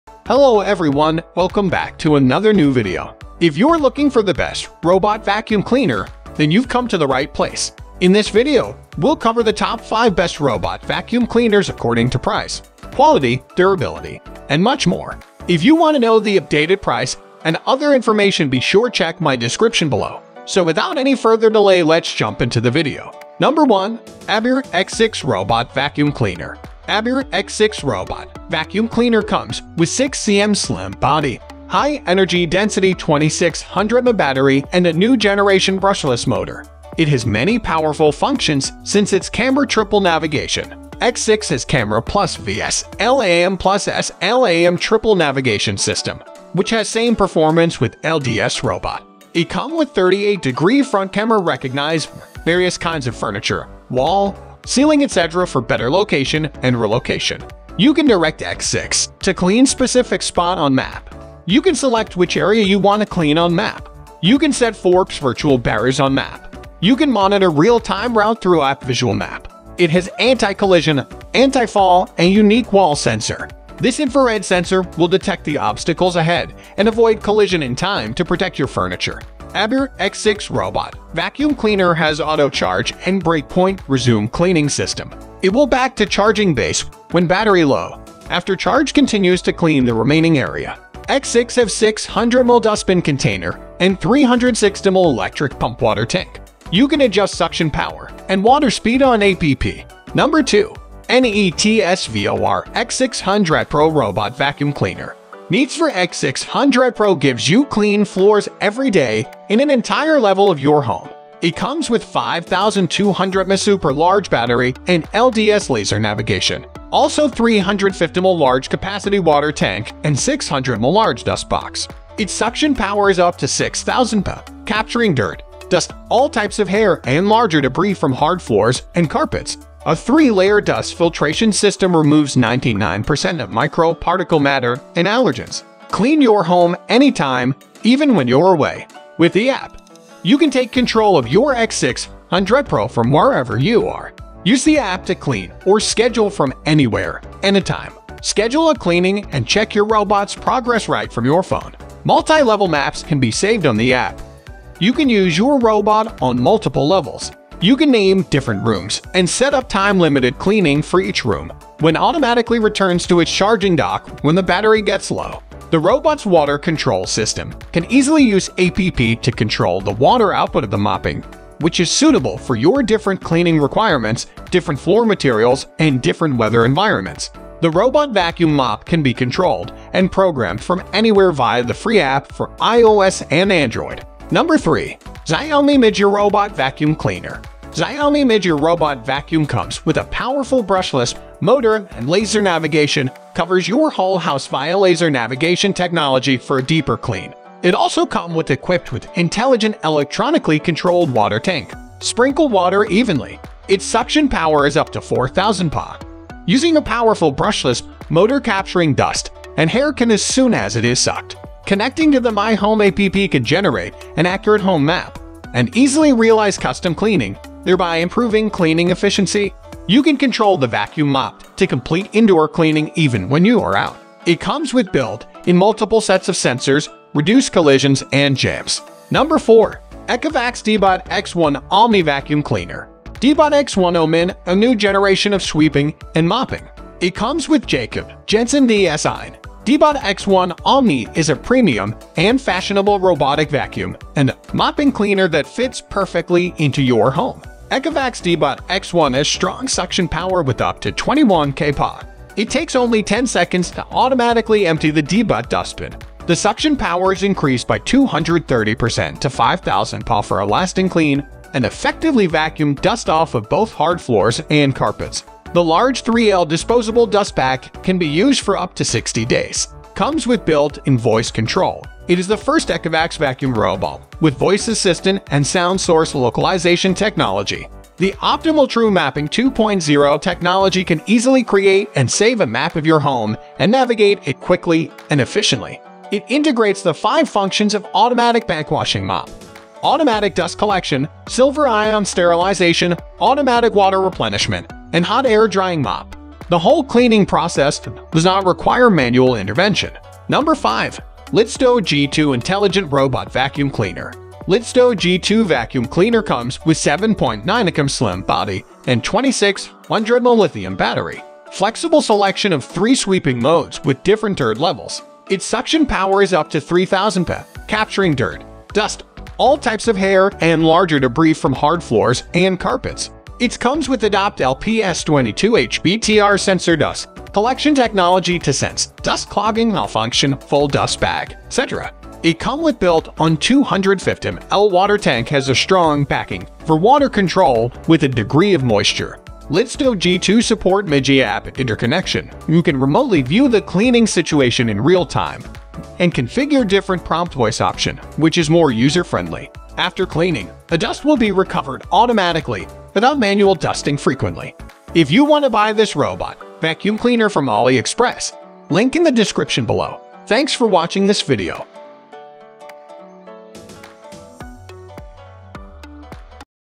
TOP 5 Best Robot Vacuum Cleaner 2022-2023
TOP 5 Best Robot Vacuum Cleaner in 2022

Looking for the best Robot Vacuum Cleaner? Check out our top picks for the best Robot Vacuum in 2022. We've reviewed the top products on the market to help you make an informed decision.

Best Robot Vacuum Cleaner 2022 AliExpress

5. Lydsto G2 intelligent Robot Vacuum Cleaner

4. ECOVACS DEEBOT X1 OMNI Vacuum Cleaner

3. XIAOMI MIJIA Robot Vacuum Cleaner

2. NEATSVOR X600pro 6000pa Robot Vacuum Cleaner

1. ABIR X6 Robot Vacuum Cleaner

Chapters
0:00 - Intro
0:26 ABIR X6 Robot Vacuum Cleaner
1:44 NEATSVOR X600pro 6000pa Robot Vacuum Cleaner
3:07 XIAOMI MIJIA Robot Vacuum Cleaner
3:32 ECOVACS DEEBOT X1 OMNI Vacuum Cleaner
5:04 Lydsto G2 intelligent Robot Vacuum Cleaner